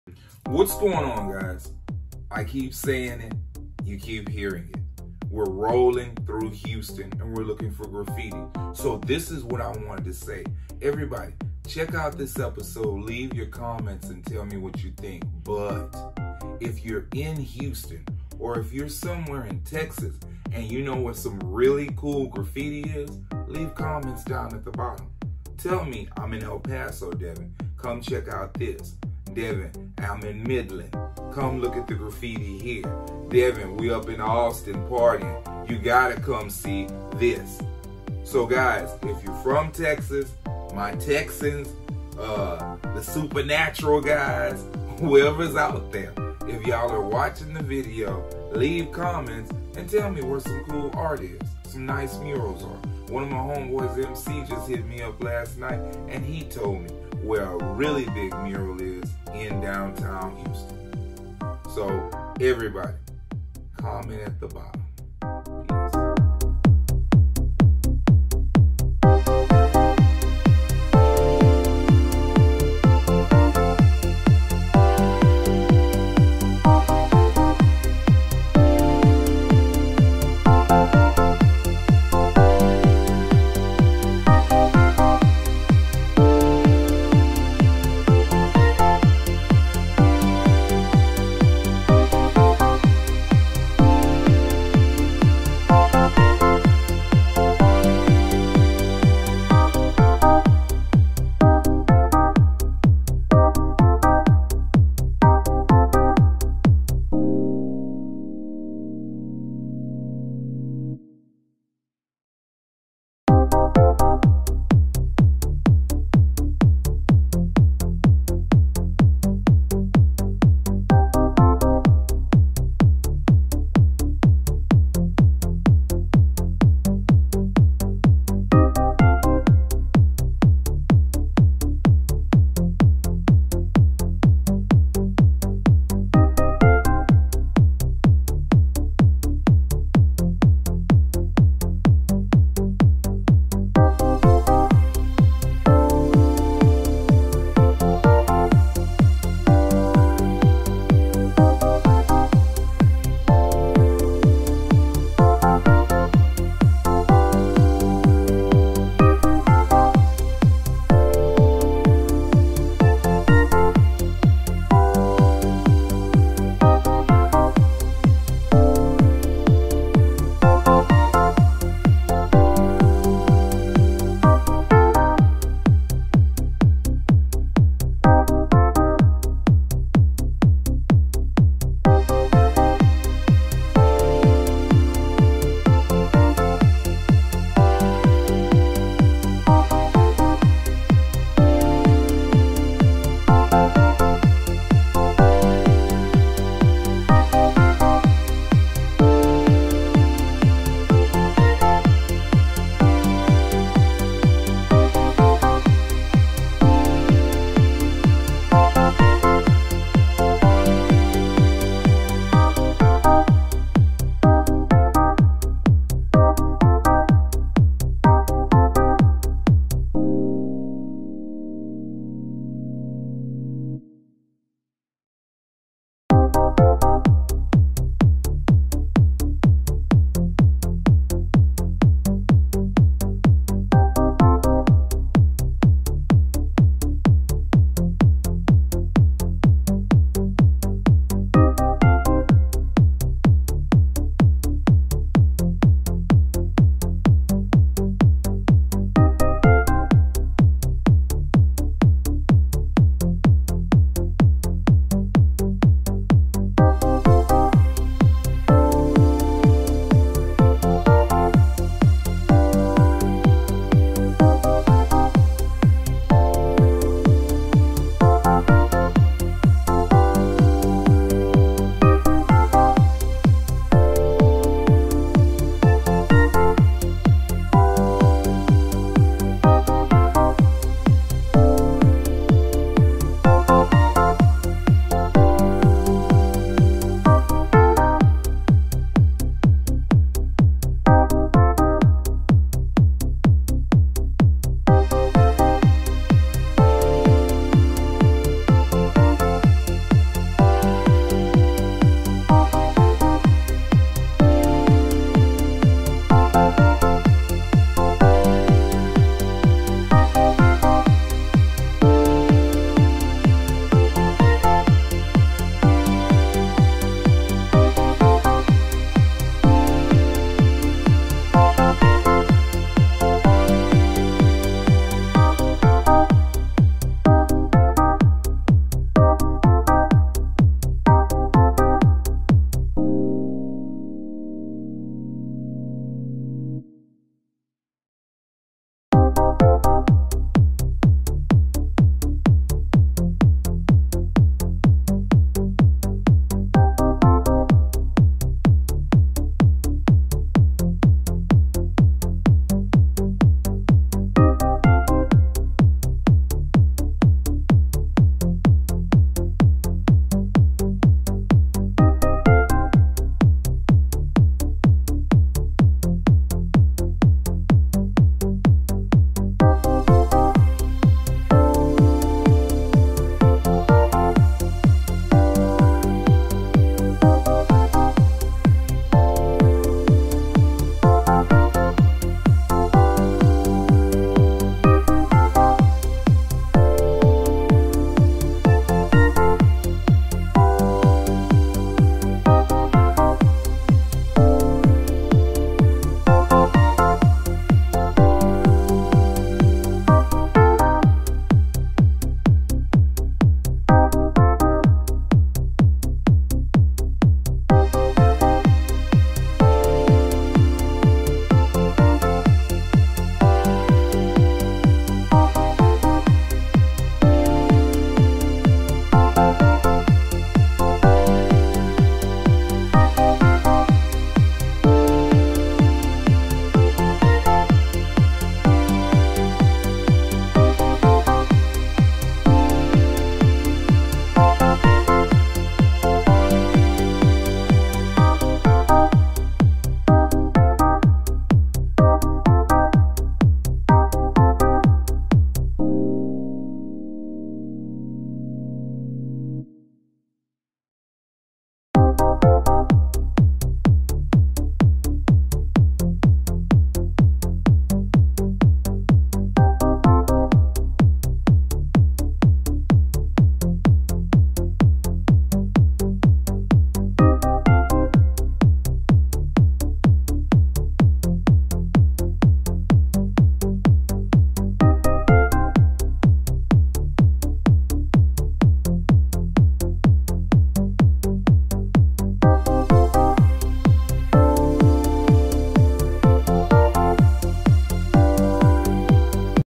This is Houston | Looking for Murals with my Family part 3
We’re driving around Houston looking for murals.